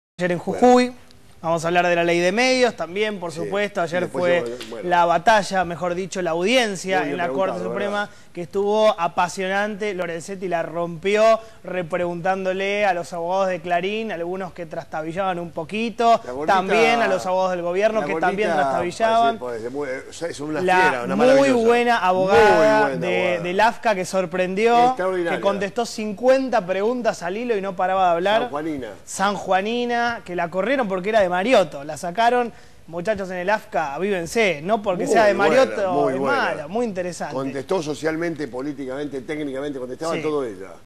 Mauro Viale fan de Graciana
Mauro Viale y Jonathan Viale elogian el gran trabajo de Graciana Peñafort Colombi en la audiencia de la ley de medios.